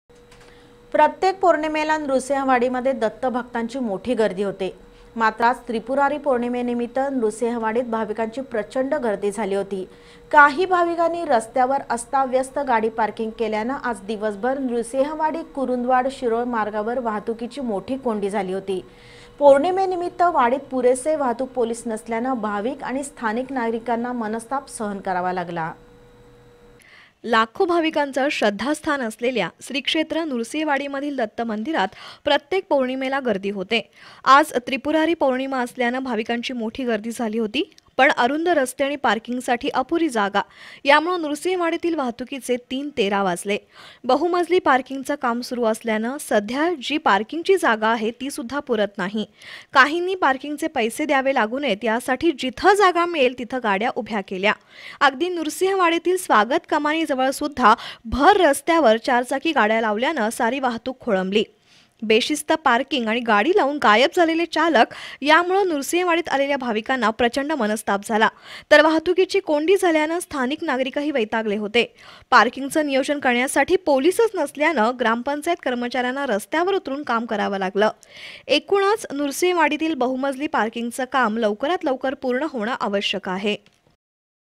वाहनधारकांचा बेशिस्तपणा आणि भाविकांची प्रचंड गर्दी यामुळं नृसिंहवाडीत प्रचंड वाहतूक कोंडी
अपुरी जागा, वाहनधारकांचा बेशिस्तपणा आणि भाविकांची प्रचंड गर्दी यामुळं नृसिंहवाडीत प्रचंड वाहतूक कोंडी, त्रिपुरी पौर्णिमेनिमित्त दत्त दर्शनासाठी भाविकांची झुंबड
Channel B